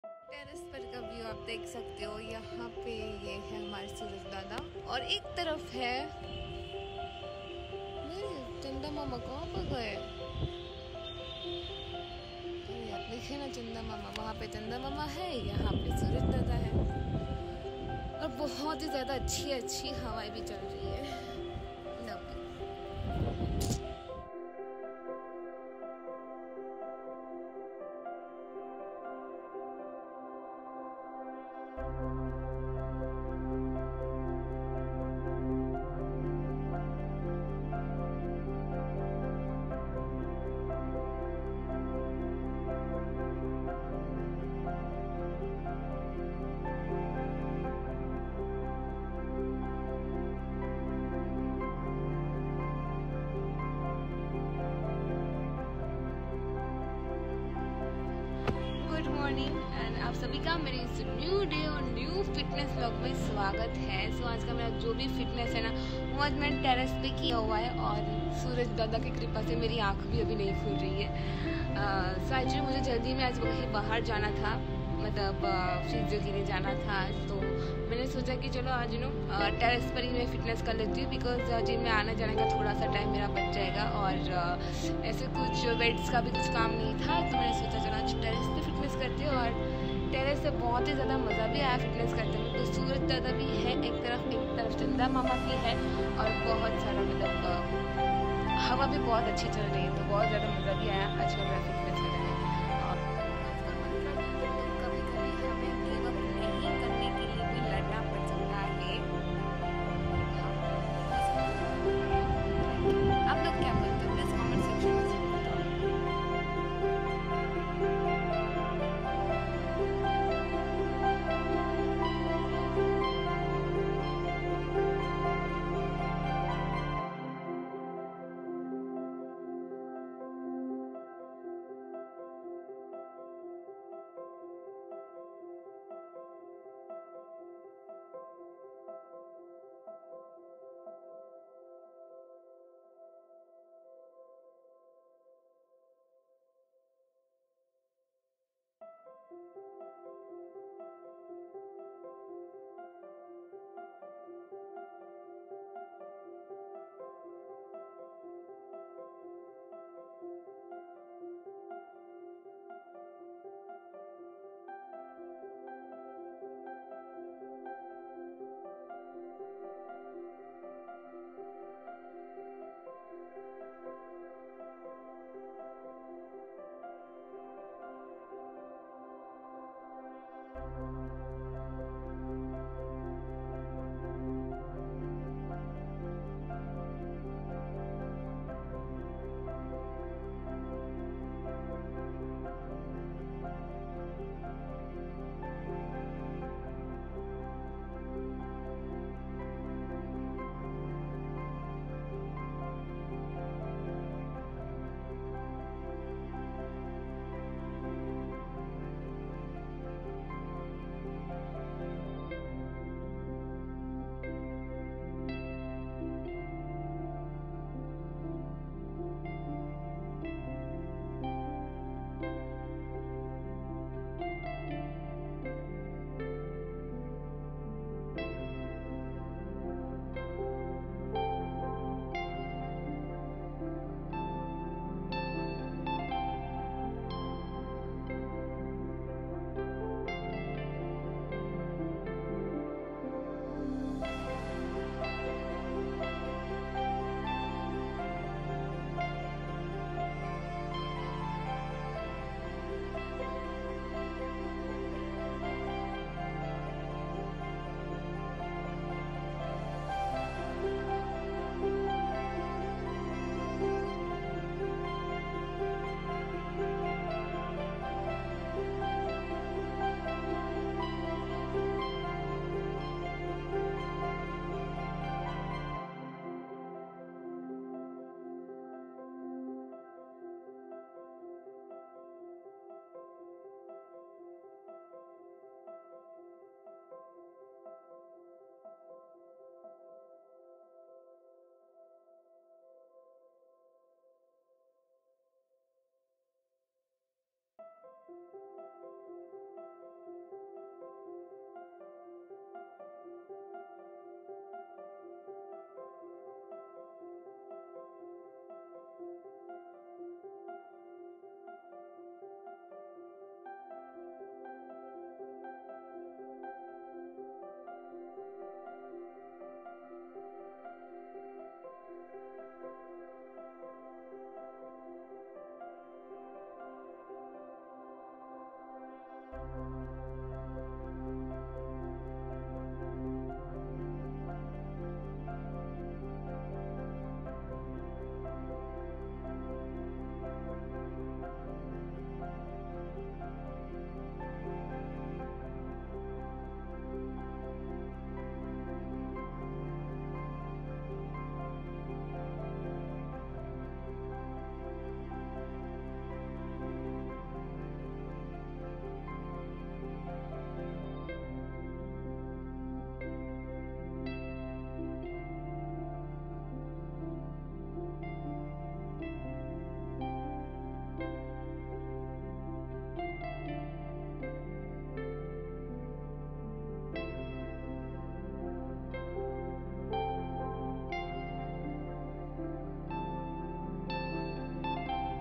Day:-19 | Home workout | Fitness vlog
Hello,  welcome to day:-19

Today I have done Core circuits

Warm up
1) stretching
2) co-ordination
3) jog (7 minutes)

Main workout

1)Half crunches            Note:-  30sec * 2set
2)Full crunches                          25 sec * 2 set
3) full ( pahlvaan) squats.         15 sec * 2 set
4) leg raises                                10 sec * 2 set
5) Mountain climber
6) High knees
7)Reverse crunches

Cooling down

Rest between exercises  10sec
Rest between set :- 30 to 40 sec